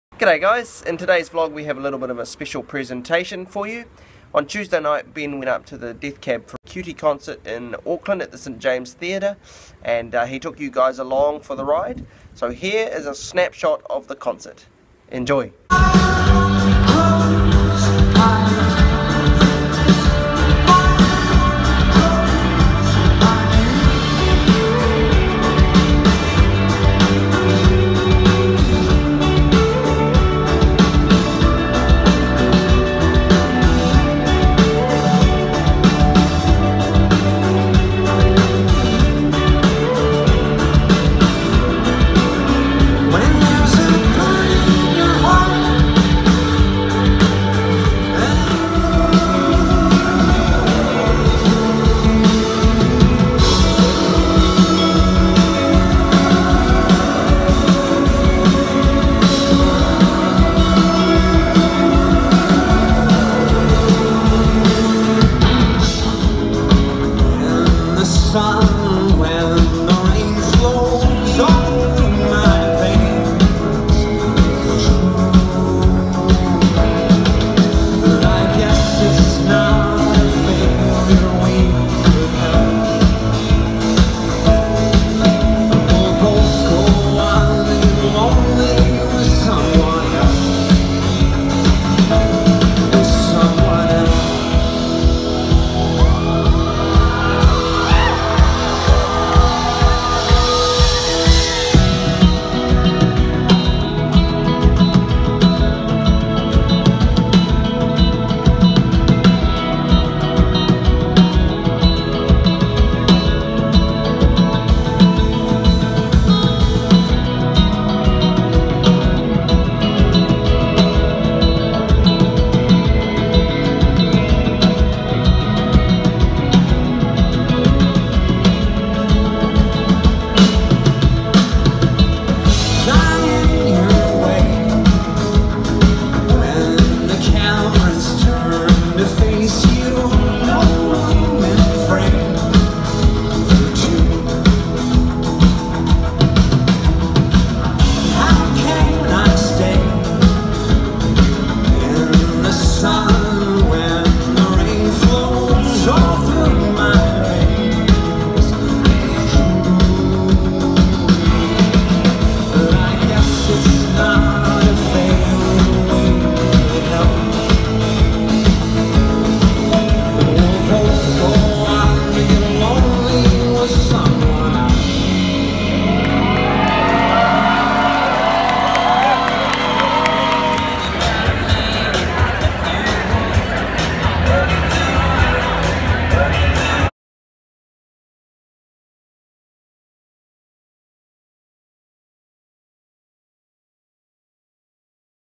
DEATH CAB FOR CUTIE'S FIRST LIVE SHOW IN AUCKLAND NEW ZEALAND! | Project 29 - VLOG 55
ADW Ben and Jay present Project 29, where Jay daily vlogs the last year of his 20s during 2016.

Death Cab for Cutie's first ever show in in Auckland, New Zealand. The show was at the St James and they were amazing!